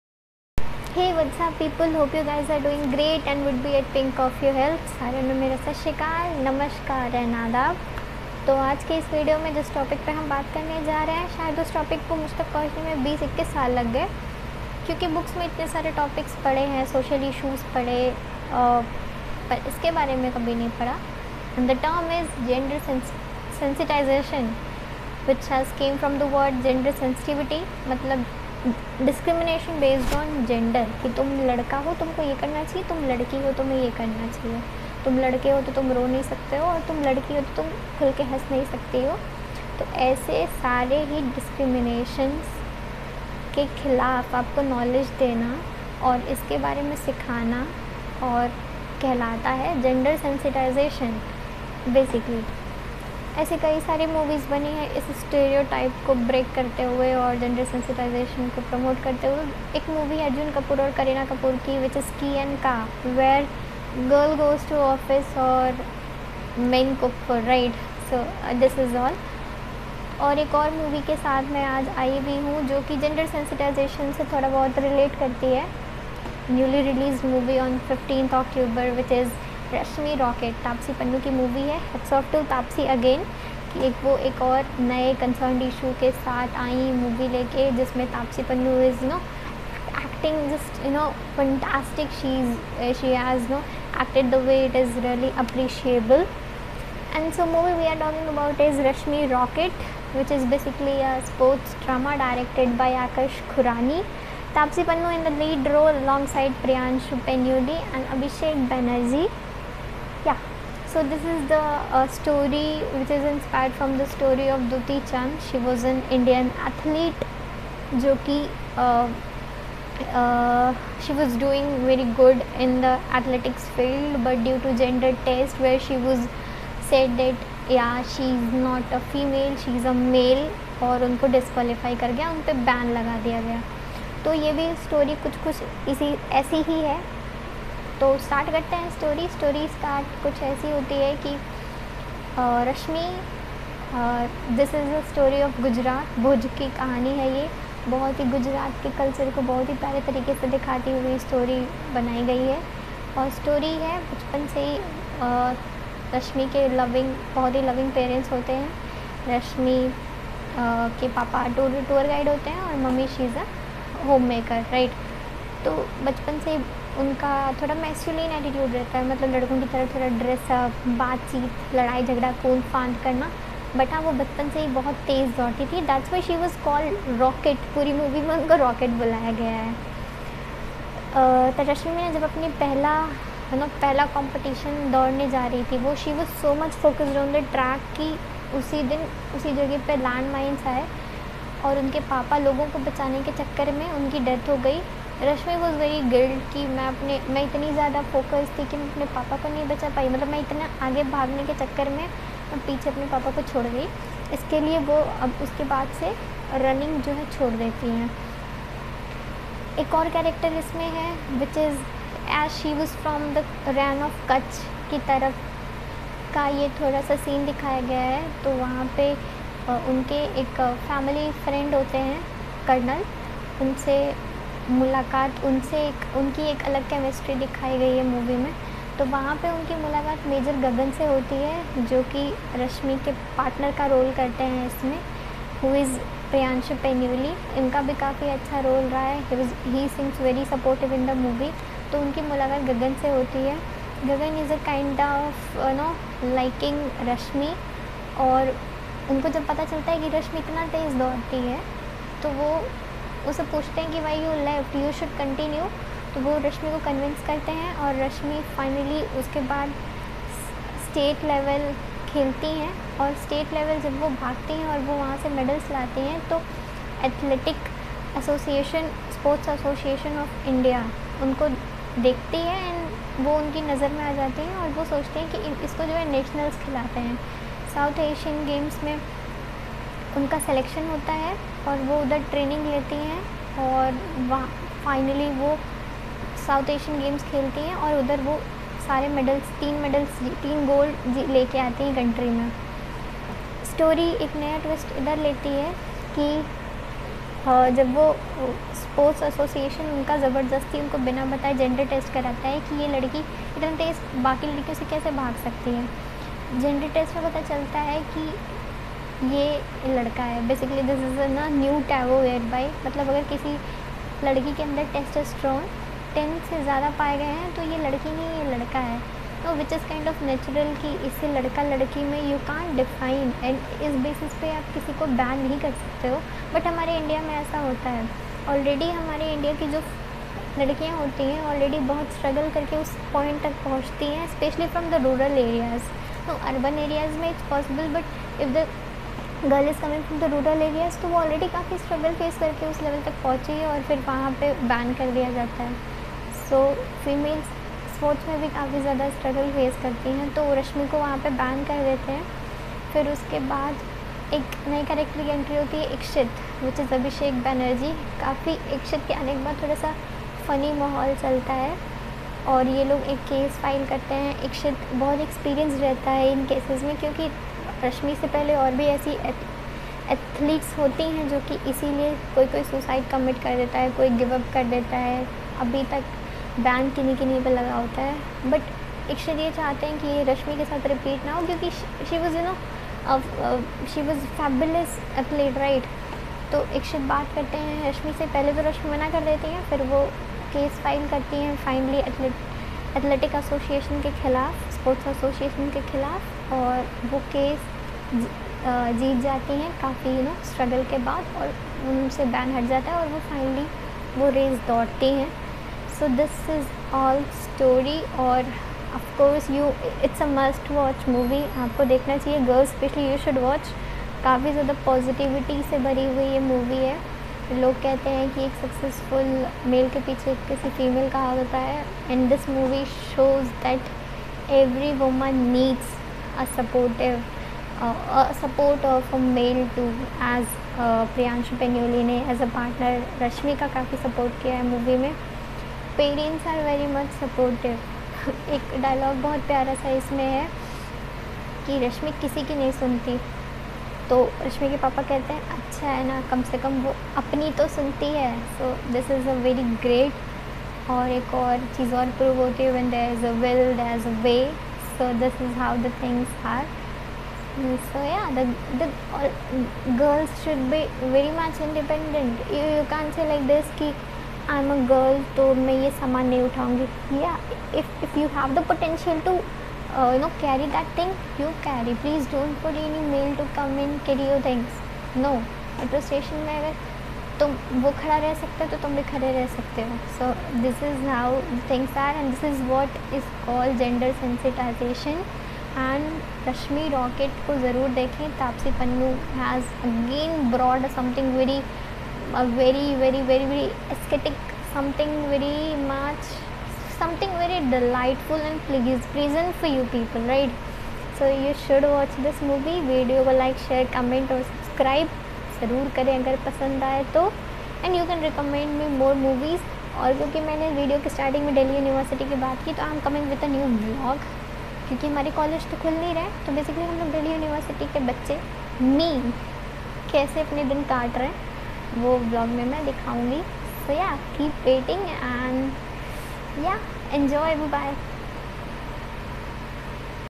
Rashmi Rocket 2021 Full Movie || Story Review || Gender Sensitisation || Tapsee Pannu
Gender Sentisation is very concerned topic, it needs promotions so that most of the voilence and descrimination against women and men can be ishewed.

Rashmi Rocket is Hindi Language sports drama inspired from the story of Dutee Chand, an Indian Athelete who was banned after gender test. It's an amazing movie came with an new and important issue related to females and offcourse every sportsperson in our country.
Must watch the movie and don't forget to give your reviews.